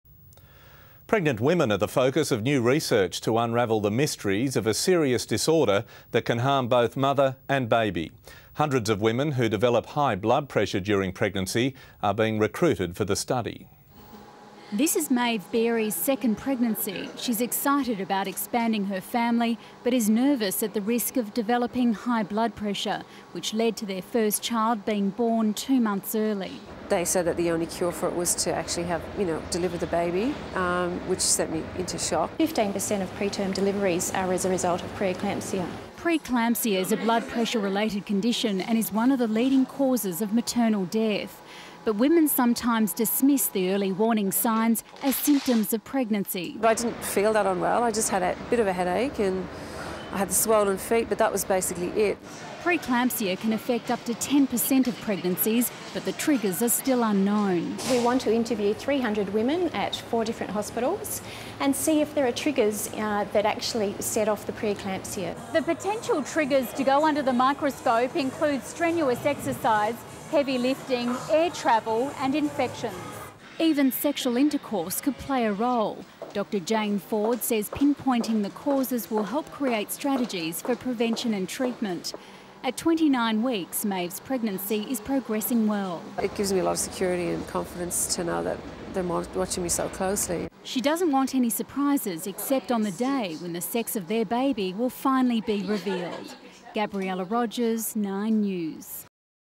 Kolling Pre eclampsia Story on Channel 9
Pre-Eclampsia new story at Kolling Institute of Medical Research.